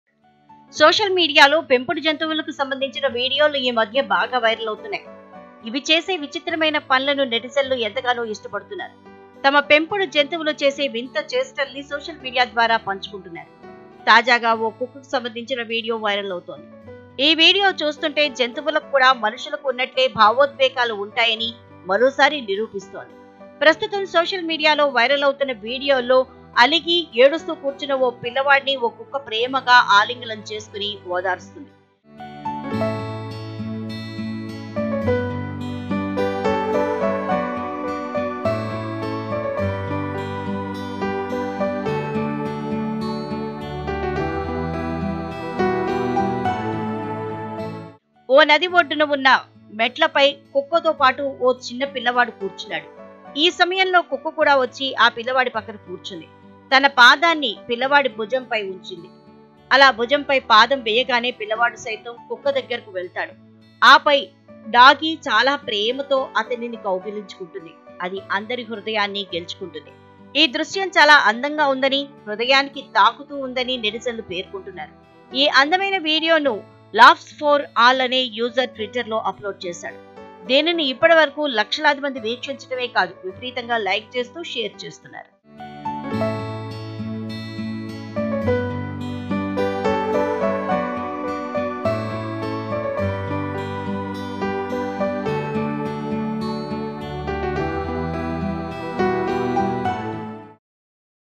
Viral Video : అలిగి ఏడుస్తున్న చిన్నారిని ఓదార్చిన శునకం..! - TV9
Credit: NewsUpdates | Rajeswari /Producer | Shiva /Editor